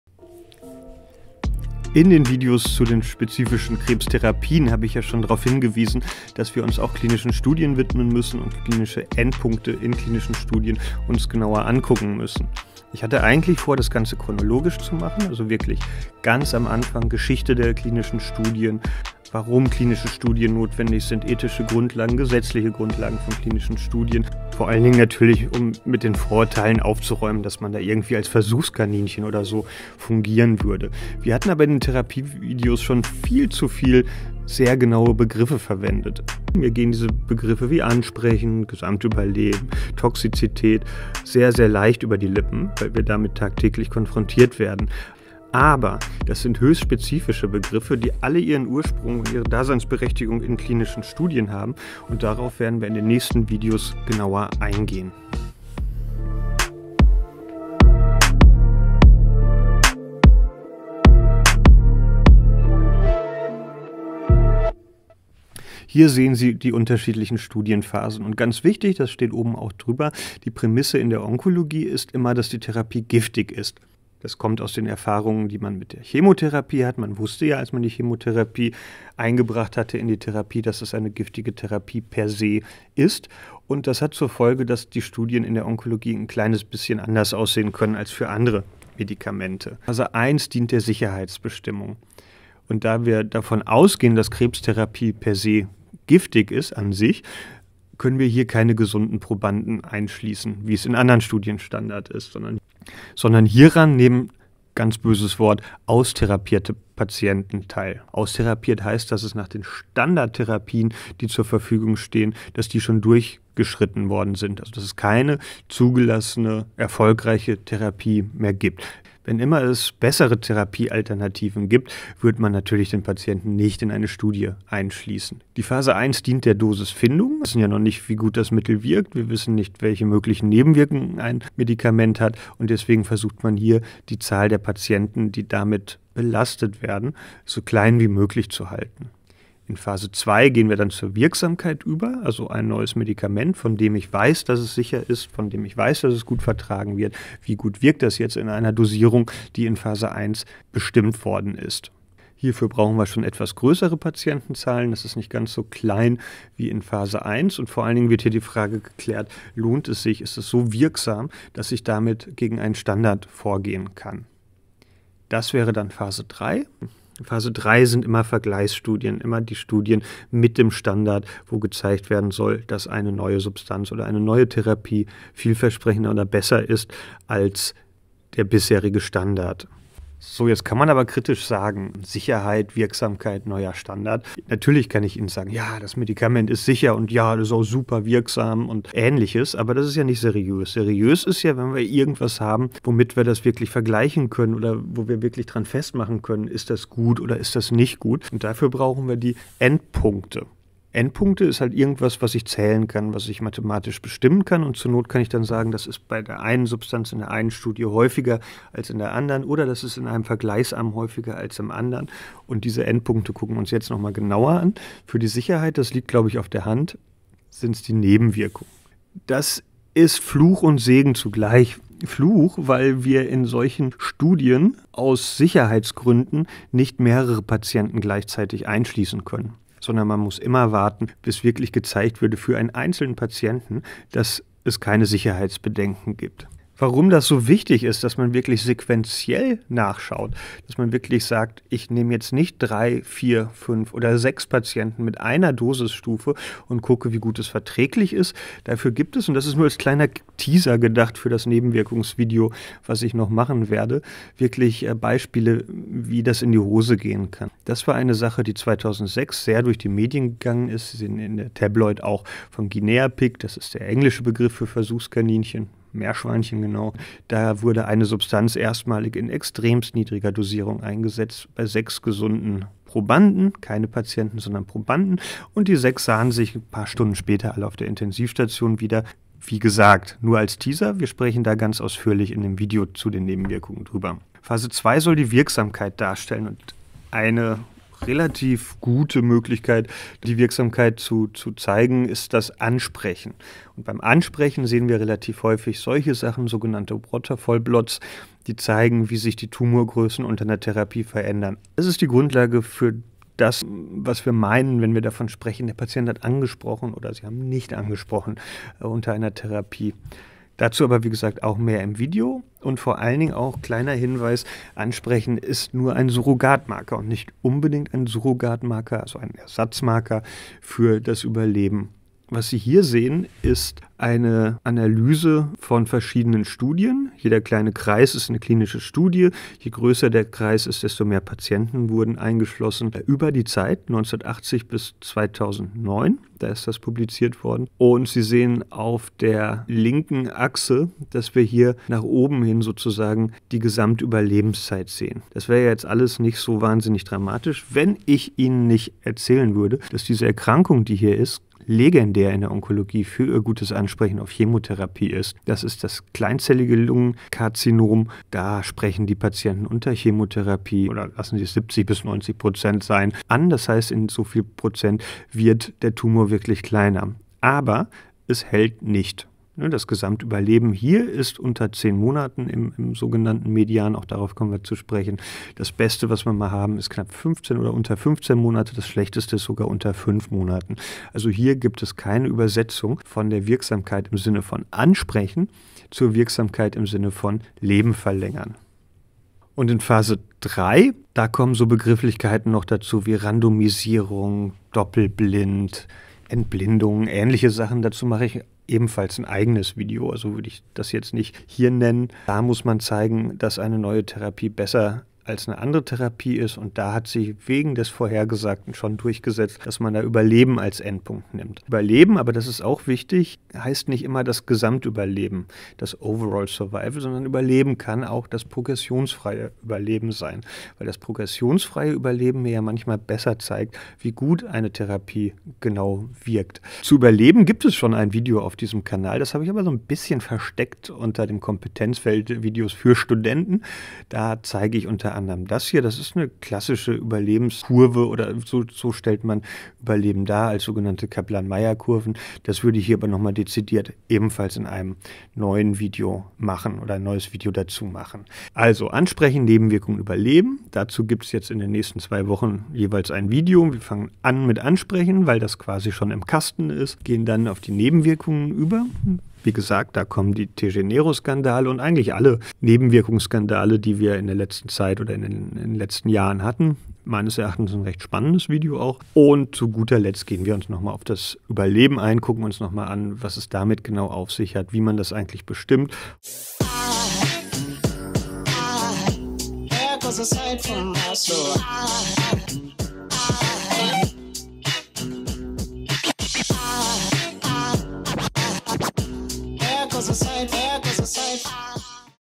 Klinische Studien: Phasen und Endpunkte
Eigentlich wollten wir uns den Videos zu klinischen Studien "vorsichtig" annähern - Geschichte der klinischen Studien, Krebstherapie-Studien, deutsche Gesetzgrundlagen... Da wir aber bereits in den anderen Videos v. a. zur Krebstherapie sehr spezifische Begriffe verwendet haben, gerade hinsichtlich der Endpunkte!, schieben wir eine kleine Videoserie hierzu ein.

Das aktuelle Video zeigt die verschiedenen Phasen der klinischen Studien, ins Detail wird dann in den Folgevideos gegangen.

Kommentare und Anmerkungen sind sehr willkommen!

Es sei auch nochmal auf unser Glossar zum Kanal verwiesen, welches aktuell Urlaub hat, ansonsten aber sukzessive erweitert wird:

Erstellt mit: Microsoft PowerPoint
Editiert mit: Final Cut Pro
Artwork: HealthCareSocialMedia